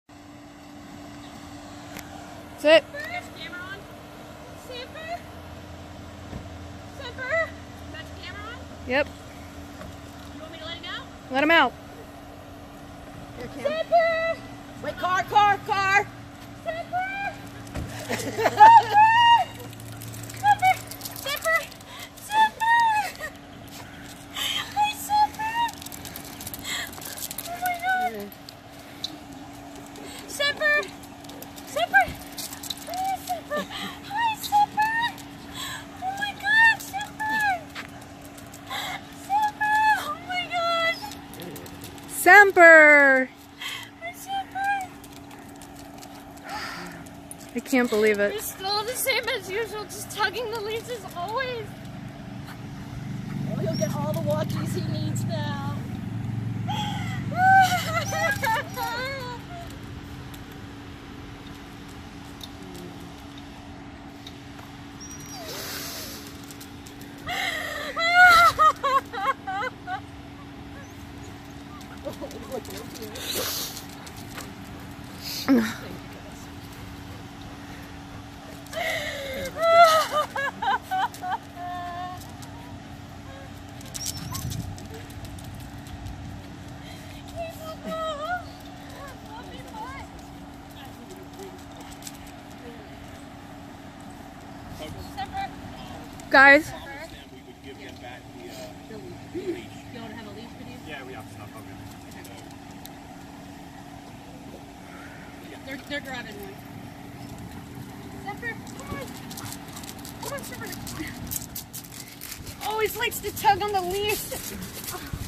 Girl who beat cancer reunited with stolen dog almost 3 years later.
We help people and sometimes their lost / stolen animals too! Here is a wonderful reunion video between our client and her dog. They were separated almost 3 years after he was stolen and sold on Craigslist!

* shared with permission from the owner. Florida Agency

Watch at your own risk of crying immediately. Here is the video and audio from Kameroun Mares reunion with her dog Semper today. 😭💙 and Ana Campos Investigations - South Florida.

Please keep in mind Kameroun is just 26 years old. She was 23 when this began. For a young girl to have the wherewithal and courage to hire a private investigator and attorney is remarkable. Shame on Petkey pets for causing her to suffer and spend her savings on fighting for her dog back because of the role they played. He was stolen and sold by her roommate 2 1/2 years ago.  She was told he was lost on April 30, 2016. But on April 11, 2016 the buyer added their name to his AKC Reunite "registered chip". They registered this registered chip on Petkey pets instead. Petkey pets did not bother to see if he was chipped already. A lawyer was retained. An ex-parte,  pre judgment. writ of replevin and request for a 'break order' was filed. The Judge ruled in her favor and the Sheriff's office seized the dog this morning returning him to the person who always had superior ownership rights. It was a great day.